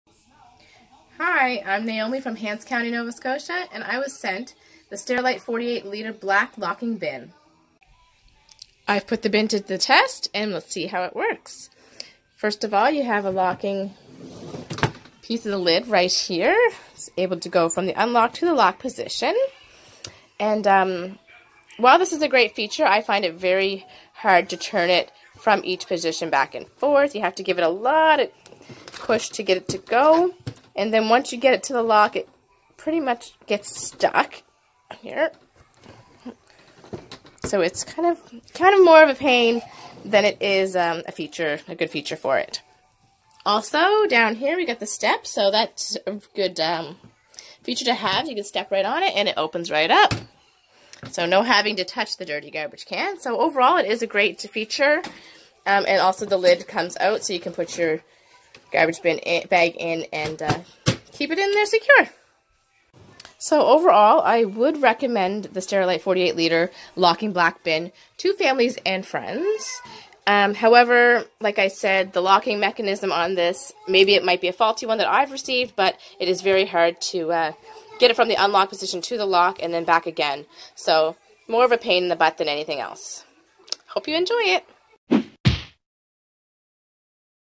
My product review: Sterilite Locking Bin
Sterilite's Locking Step-On Wastebasket is designed with a modern textured aesthetic and features a locking lid that will prevent pets and children from accessing the trash. Step-On lid opens with a slight step on the foot pedal when unlocked. Locking lid keeps pets and children out of the trash.